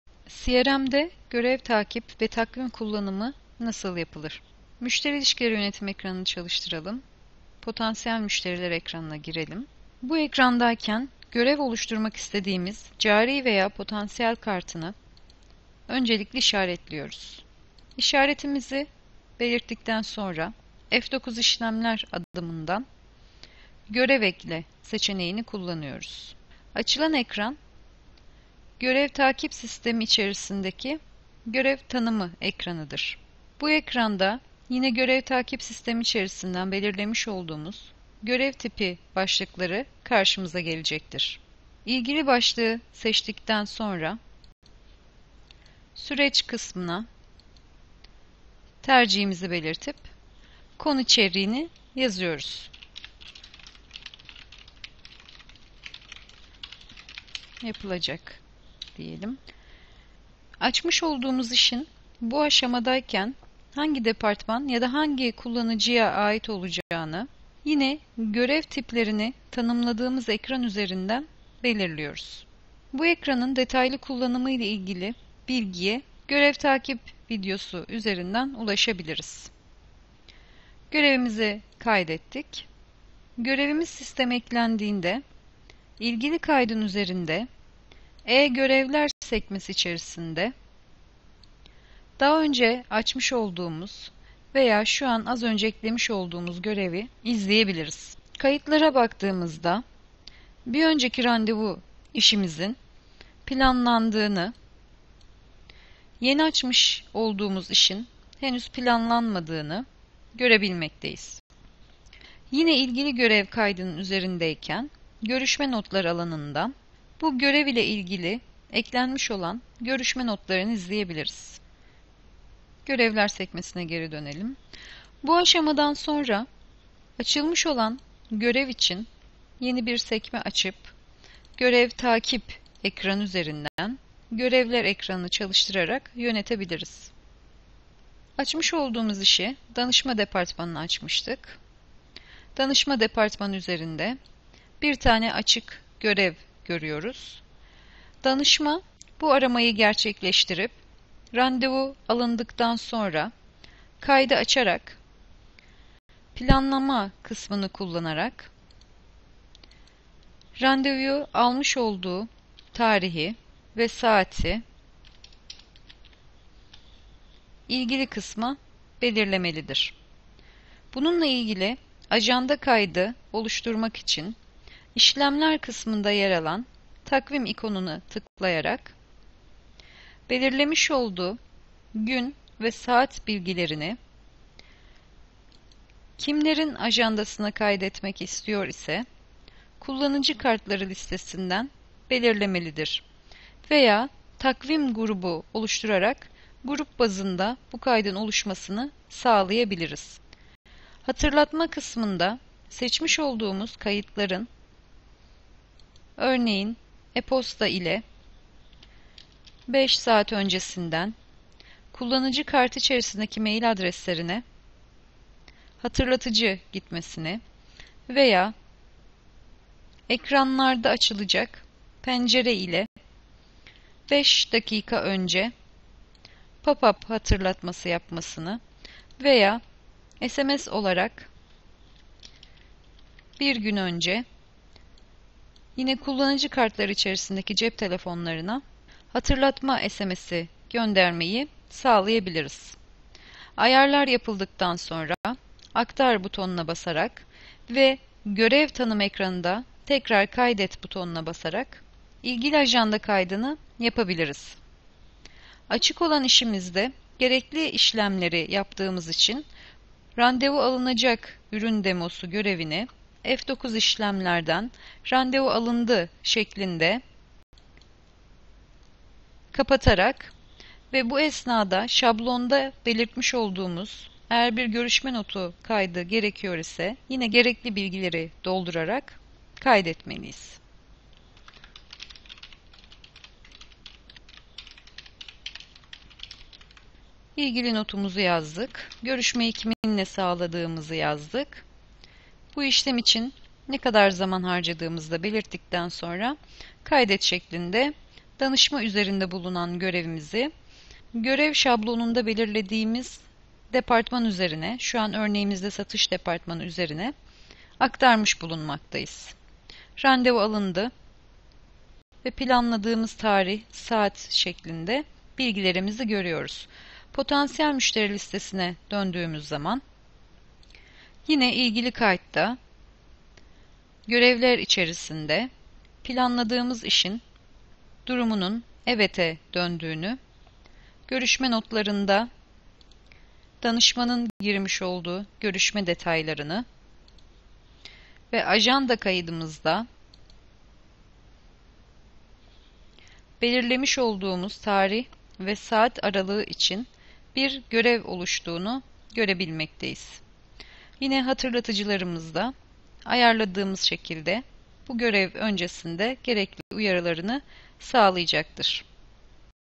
DİA Akademi - Crm Üzerinden Görevler Ve Takvim Ajandası Nasıl Kullanılır?
DİA Akademi - CRM üzerinden görevleri ve takvimdeki ajanda işlemlerini nasıl kullanabileceğini anlatan eğitim videosudur.